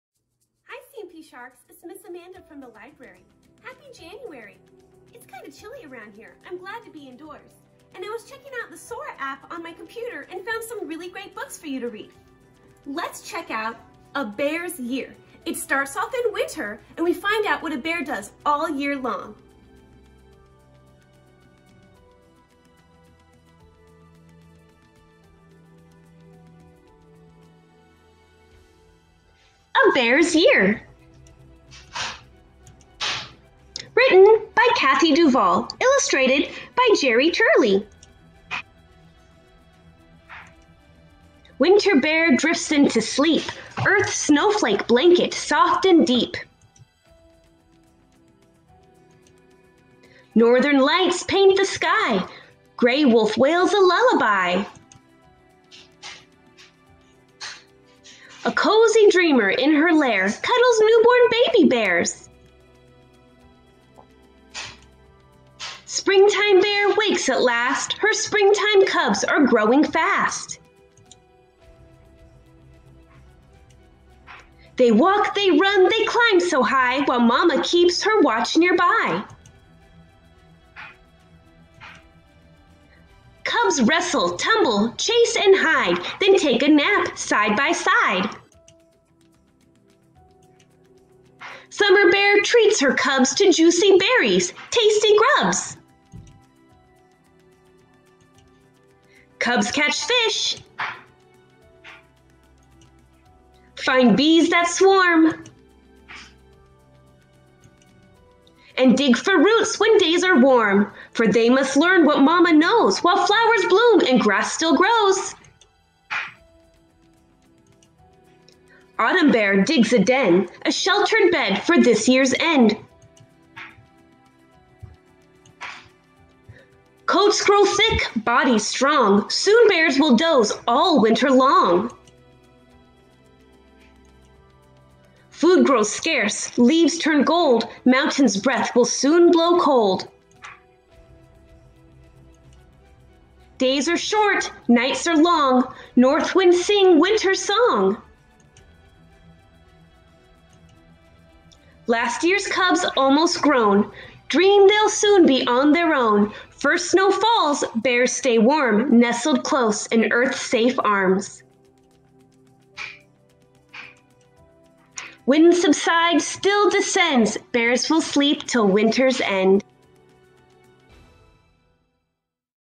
A Bear's Year
Written by Kathy Duval

Illustrated by Gerry Turley

"Deep in her den under a snowflake blanket, Mama snuggles her newborn babies. When spring arrives, the bears awaken and emerge from their lair, and as the weather wars to summer, Mama teaches her young ones to fish, gather berries, and dig for roots. Then, in fall, the leaves turn gold, food grows scarce, and the family prepares for hibernation and the coming winter."